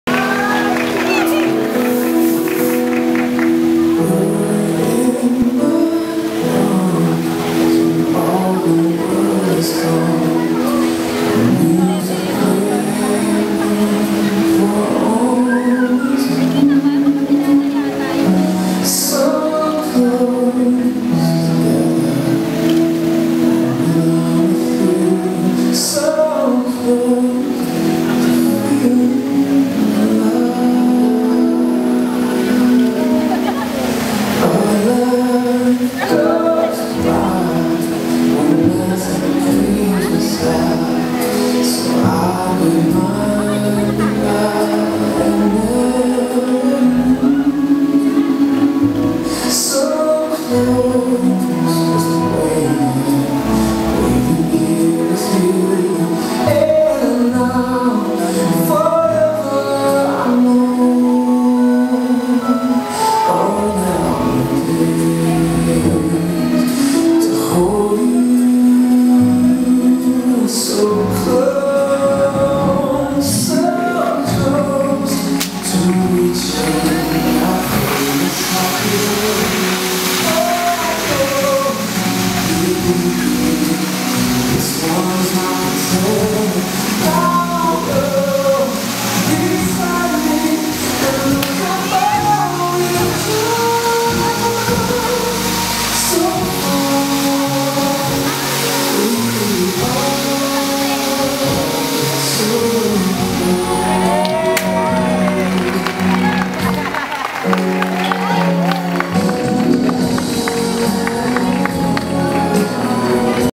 JV and Kim Calayan-Terte's Wedding Dance Number
John Vincent Terte and the former Camille Maria Ysabelle S. Calayan were wed on 6 December 2014 at Fernbrook Gardens, Paranaque City at the height of Typhoon Ruby's landfall in Samar Island. JV is the son of Gen. and Mrs. Federico and Leila Terte. Kim is the daughter of Atty. Ronaldo Antonio and Susan Soliman-Calayan. JV is a banker and Kim is a physician. Both hail from Lucena City, Philippines. The groom is an alumnus of Centro Escolar University while the bride is a graduate of University of Santo Tomas. They make such a handsome pair. Congratulations and best wishes to you two.